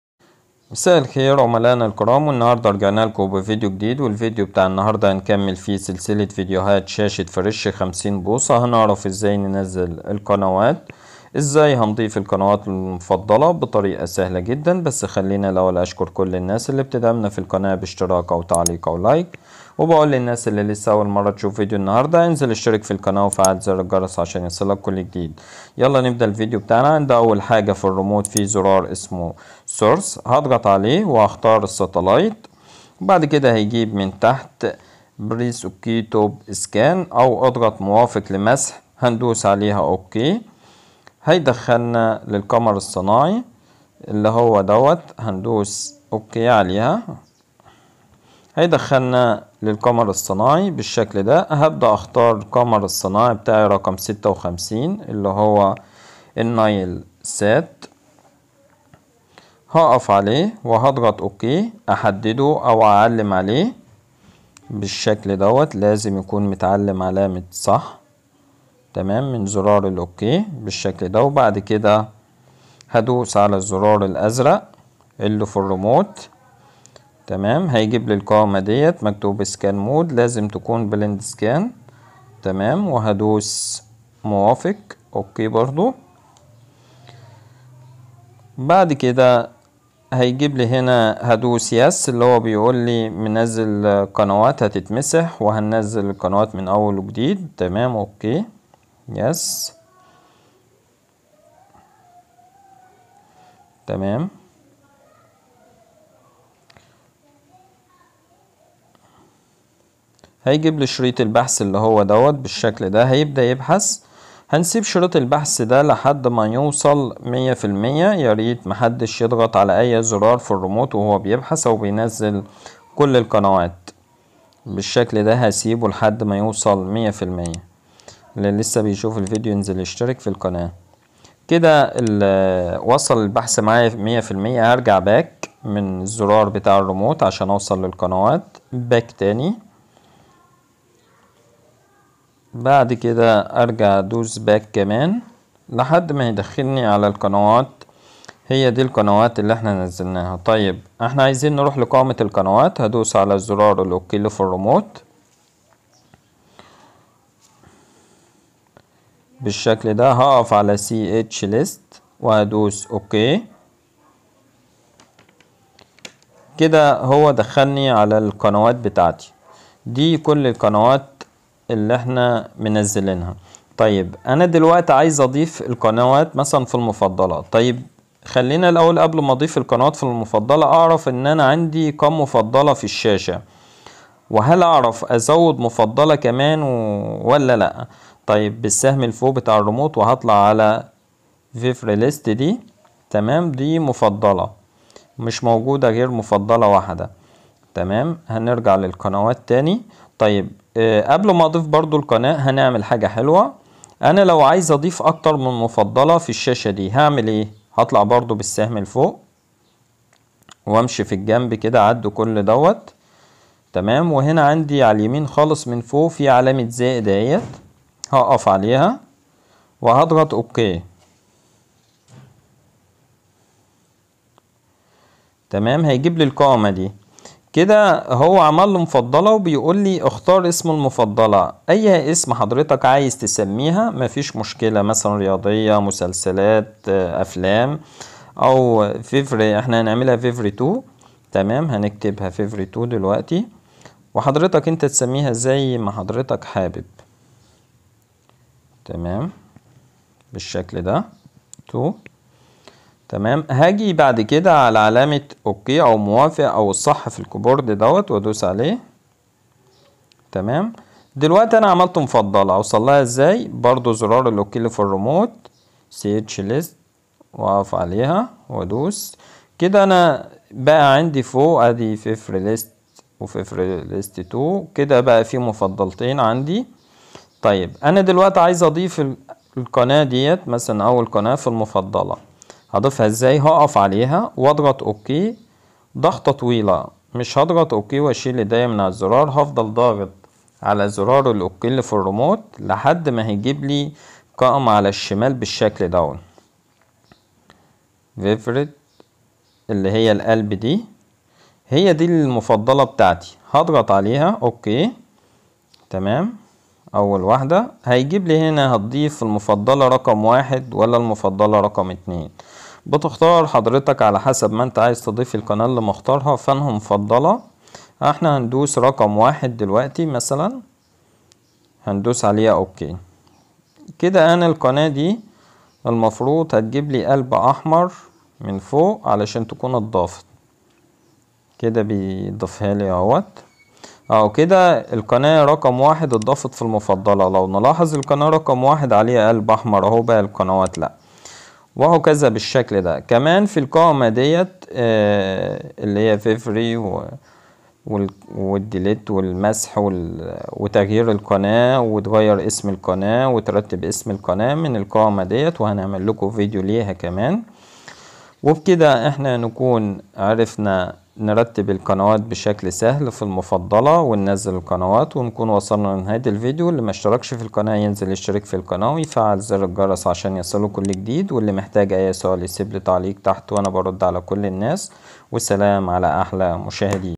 برمجة شاشه فريش اندرويد اسمارت ريسيفر داخلي وكيفية إضافة القنوات الي القائمة المفضله
شرح كامل عن برنامج شاشات فريش اندرويد اسمارت ريسيفر داخلي بطريقة سهلة جدا وصحيحة وكيفية تنزيل القنوات شاشه فريش اندرويد اسمارت وطريقة ترتيب القنوات في القائمه المفضله شاشه فريش اندرويد 50بوصه وطريقة عمل بحث صوتي شاشه فريش